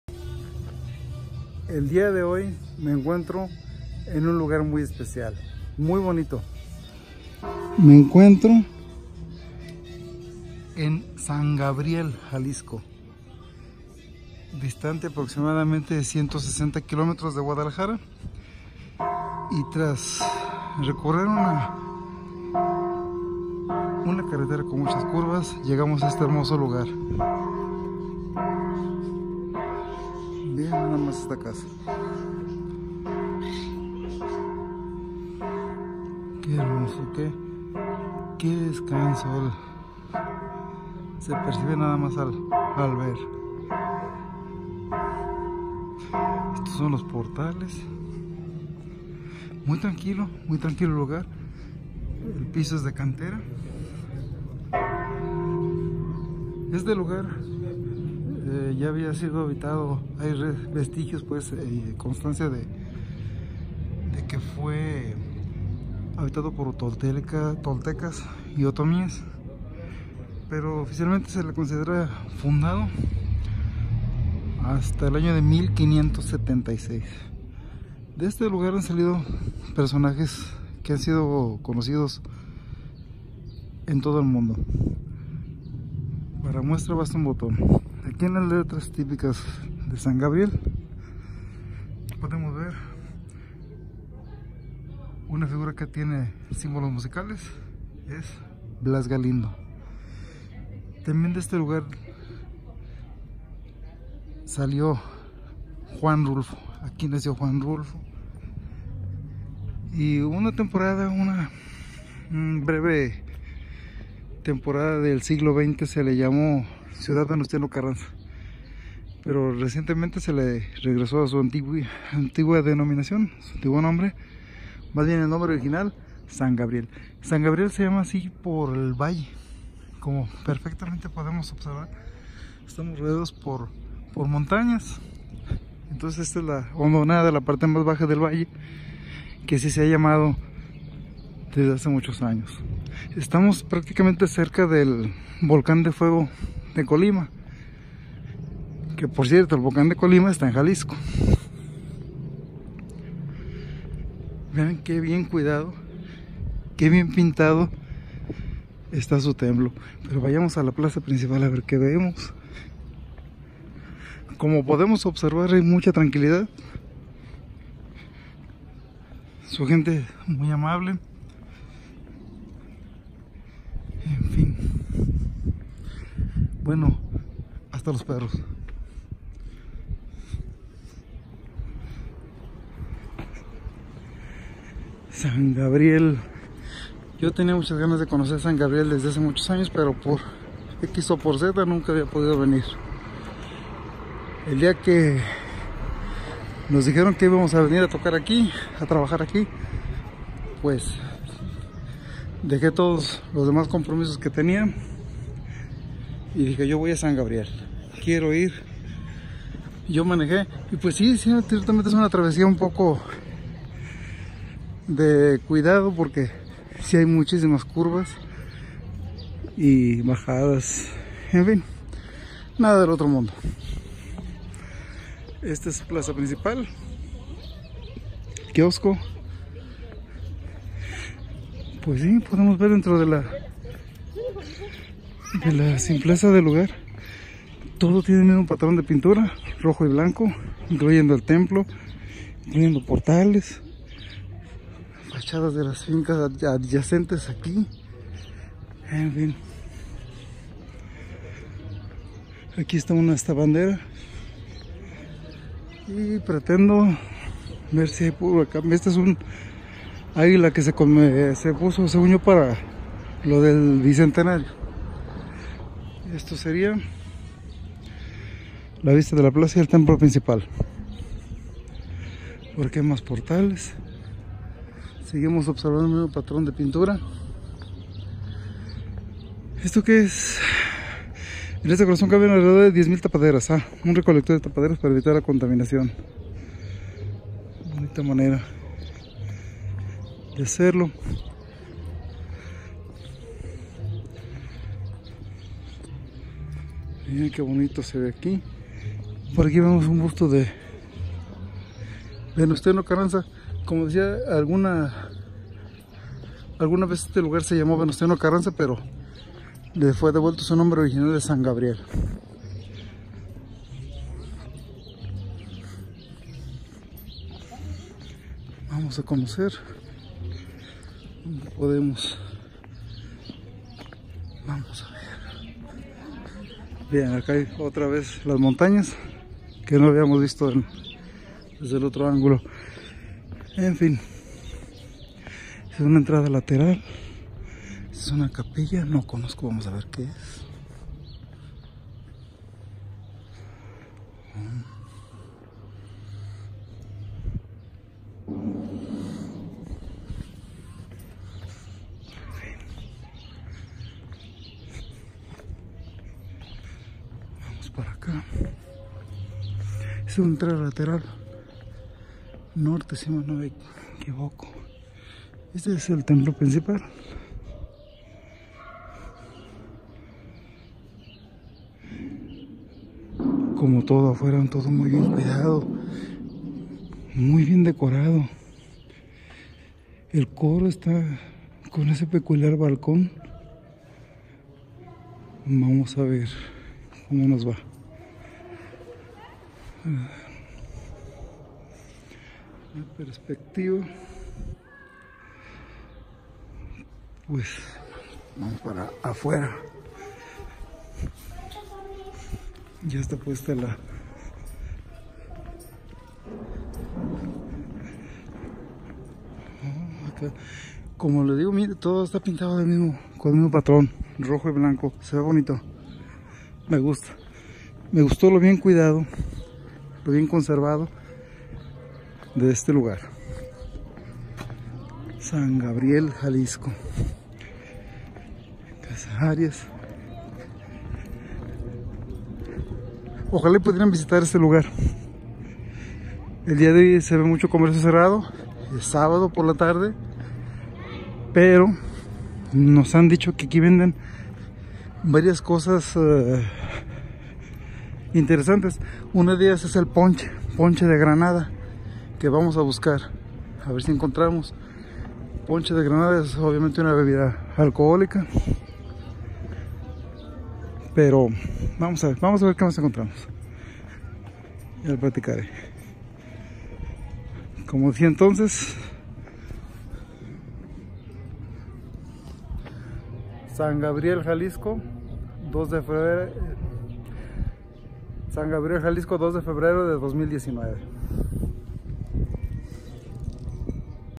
San Gabriel, Jalisco.
San Gabriel es un pequeño pueblo en la región de Amula, muy bien cuidado, limpio y con mucha historia.